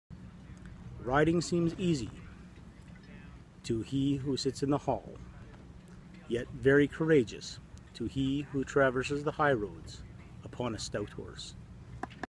Anglo Saxon Rune poem Raido
A reading from the Old English Rune poem's Rad Stanza. I study one Rune every two weeks. Starting near the Solstice with Fehu - Fe